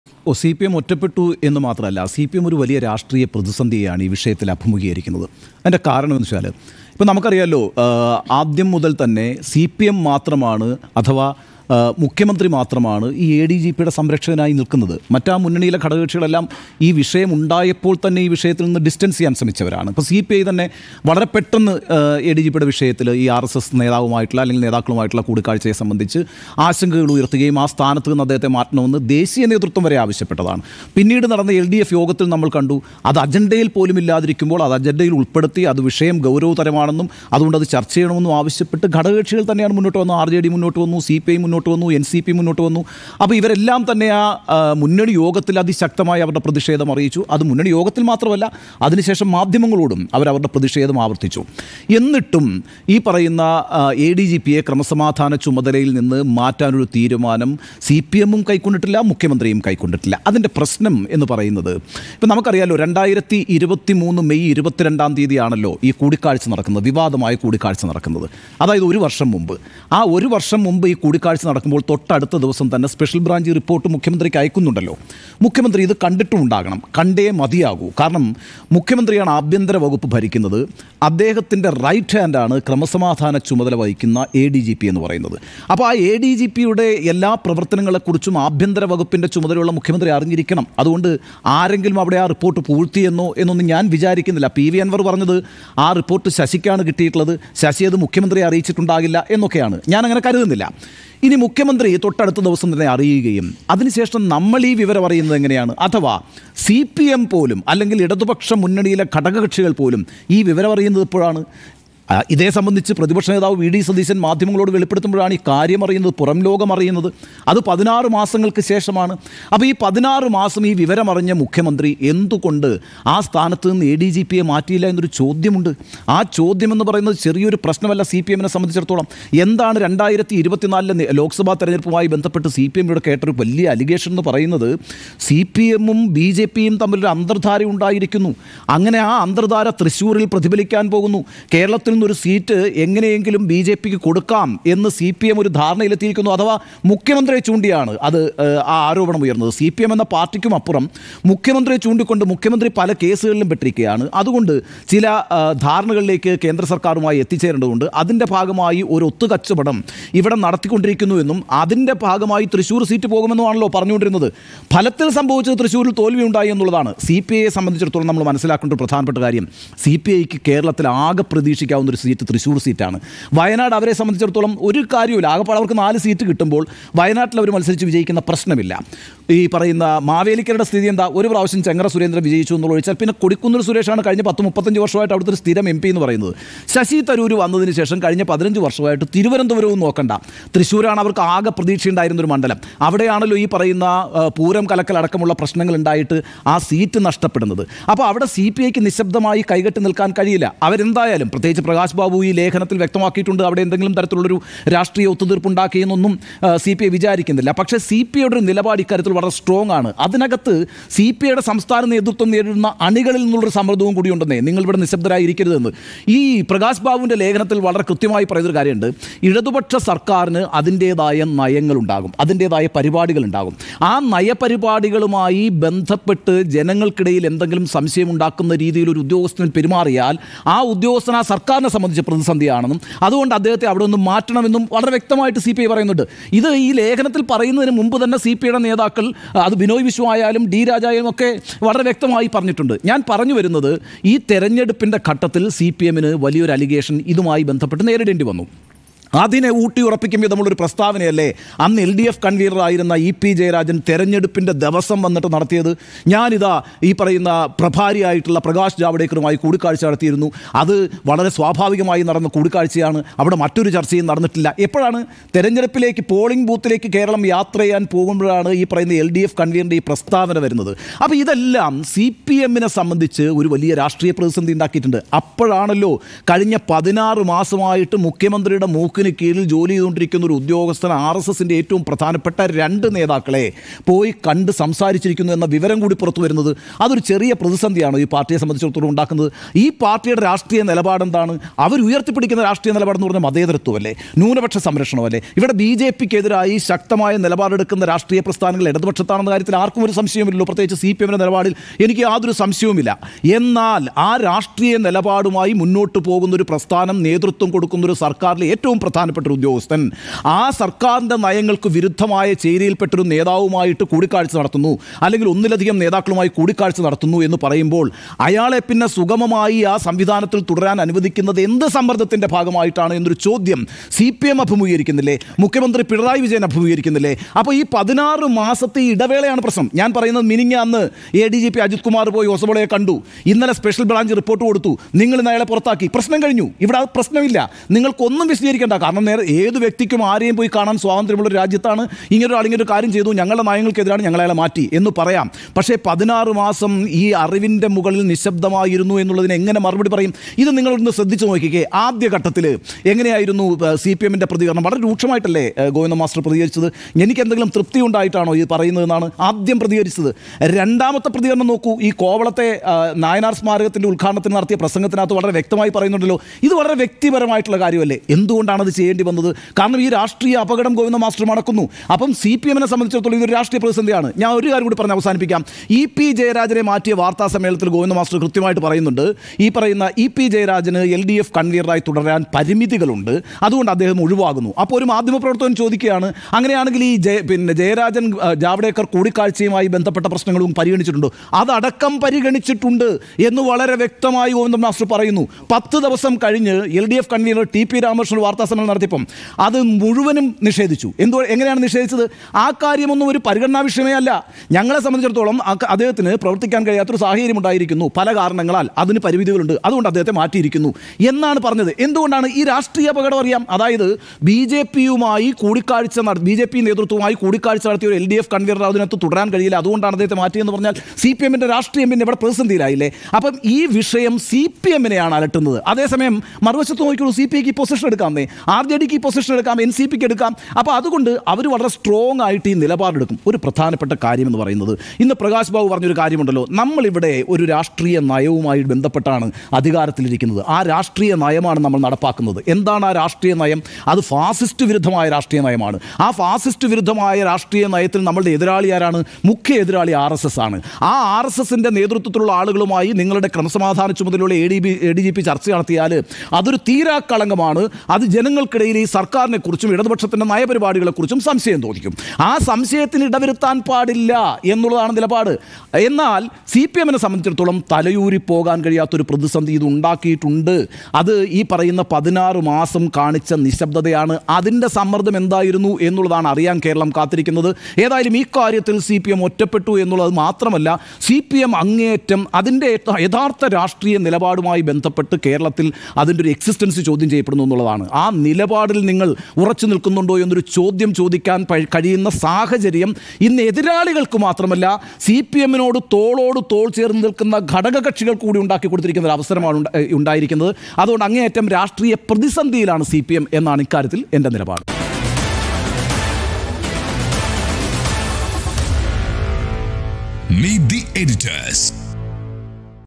അറിവിന്റെ മുകളിൽ മുഖ്യൻ നിശബ്ദനായിരുന്നു | Unni Balakrishnan | CPIM | M R Ajith Kumar
അറിവിന്റെ മുകളിൽ മുഖ്യൻ നിശബ്ദനായിരുന്നു; ഉണ്ണി ബാലകൃഷ്ണൻ | CPIM | M R Ajith Kumar

ഒറ്റപ്പെട്ടോ സിപിഐഎം? | Meet The Editors | CPIM | M R Ajith Kumar

With Regards
Team RBC